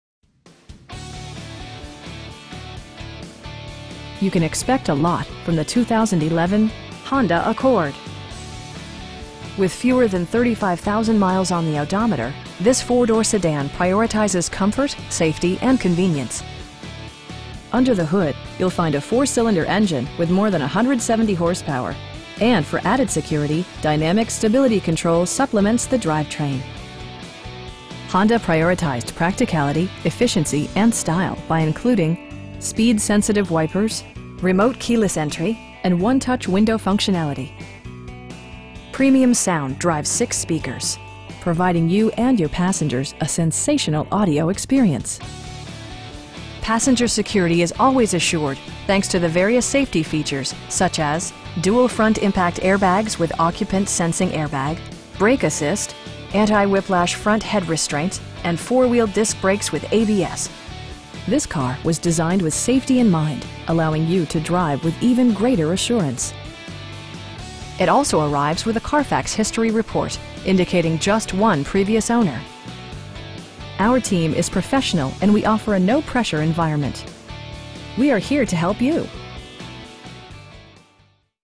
2011 Honda Accord Sdn I4 Auto LX in Gulfport, MS 39503
Pat Peck Honda
11151 Highway 49
in Gulfport, MS 39503

You can expect a lot from the 2011 Honda Accord. With fewer than 35,000 miles on the odometer, this 4 door sedan prioritizes comfort, safety and convenience. Under the hood you'll find a 4 cylinder engine with more than 170 horsepower, and for added security, dynamic Stability Control supplements the drivetrain. Honda prioritized practicality, efficiency, and style by including: speed sensitive wipers, front bucket seats, air conditioning, power door mirrors, remote keyless entry, an overhead console, and 1-touch window functionality. Premium sound drives 6 speakers, providing you and your passengers a sensational audio experience. Passenger security is always assured thanks to various safety features, such as: dual front impact airbags with occupant sensing airbag, front SIDE impact airbags, traction control, brake assist, anti-whiplash front head restraints, a panic alarm, and 4 wheel disc brakes with ABS. This car was designed with safety in mind, allowing you to drive with even greater assurance. It also arrives with a Carfax history report, indicating just one previous owner. Our team is professional, and we offer a no-pressure environment. We'd be happy to answer any questions that you may have. We are here to help you.